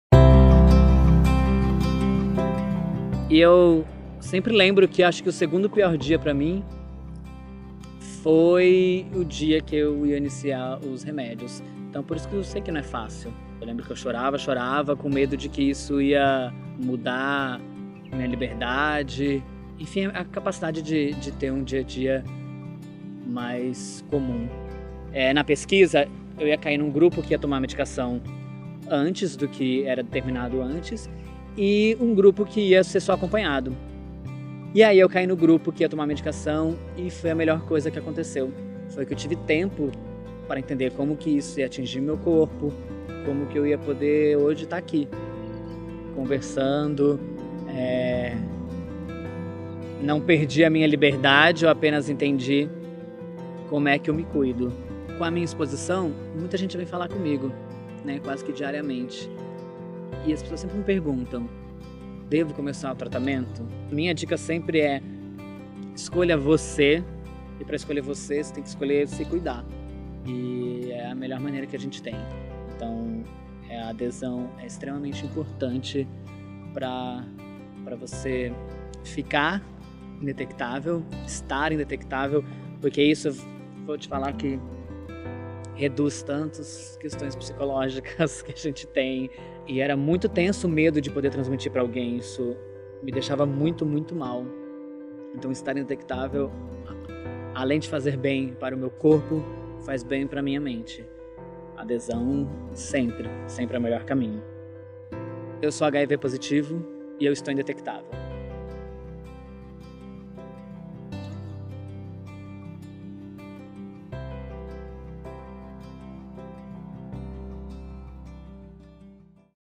CAMPANHA INDETECTÁVEL 2018 - RAFAEL BOLACHA
A Campanha “Indetectável” apresenta o depoimento do ator e produtor Rafael Bolacha, que se descobriu com HIV em 2009. Ele precisou de um tempo para entender a nova realidade, como lidar com a doença e com a sociedade. Hoje, Rafael afirma que, com a adesão ao tratamento, ficou mais confiante. Esse foi o passo mais importante para continuar sua trajetória de vida.